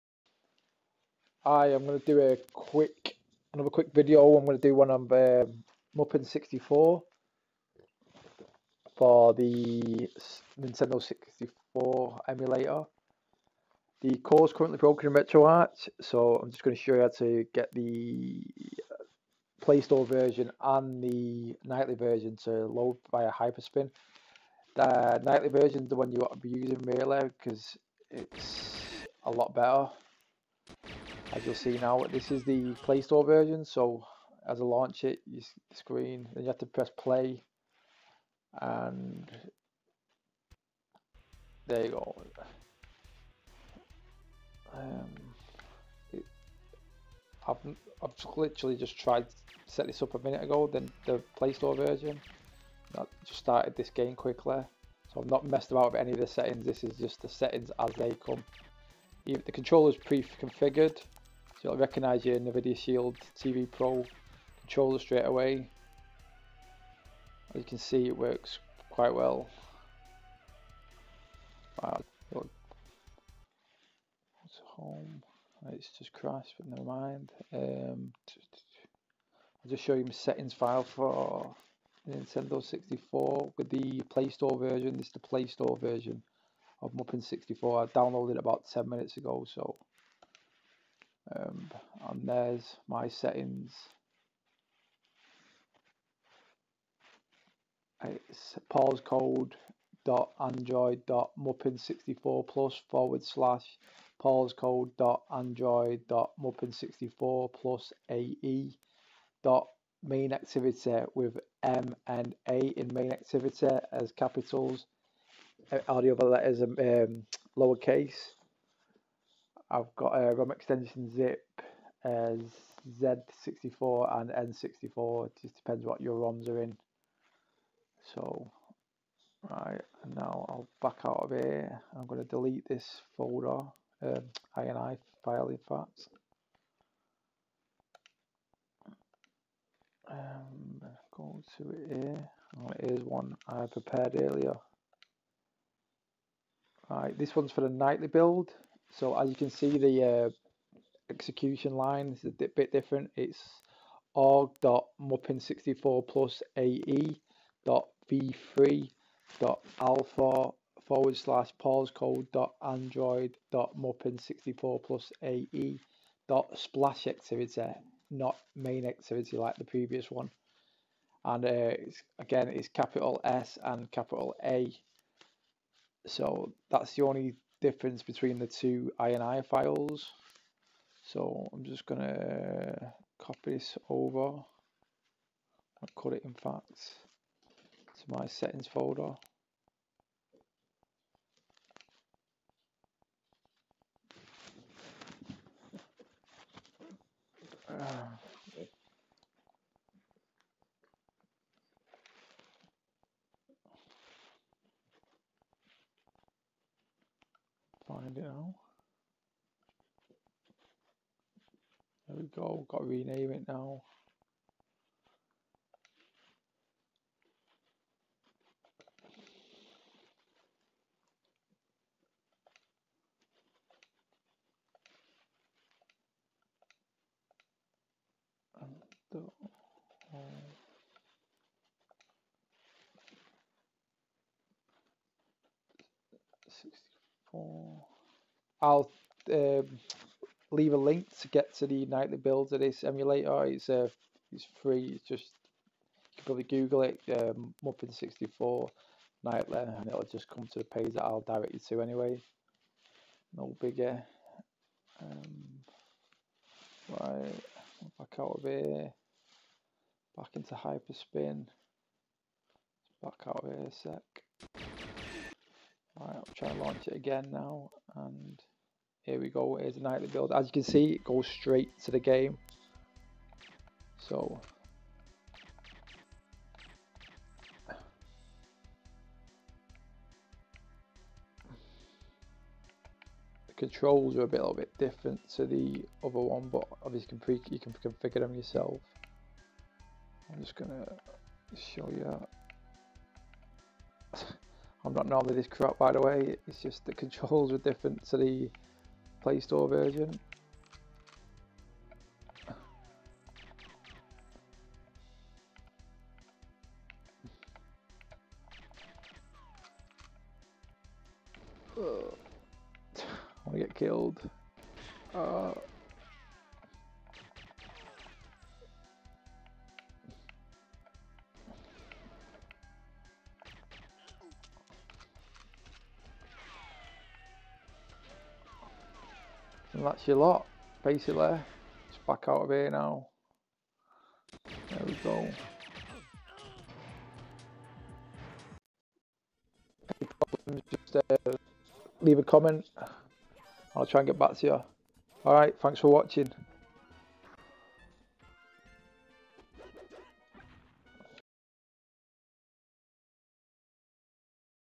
Android Hyperspin NINTENDO 64 emulator Mupen64plus and Mupen64plusae launch setup!!!
Just a quick guide to getting Mupen64plus and Mupen64ae emulators to launch through Hyperspin for Android.

Mupen64Plus AE
exe=paulscode.android.mupen64plus/paulscode.android.mupen64plusae.MainActivity
parameters=

Nightly builds
exe=org.mupen64plusae.v3.alpha/paulscode.android.mupen64plusae.SplashActivity
parameters=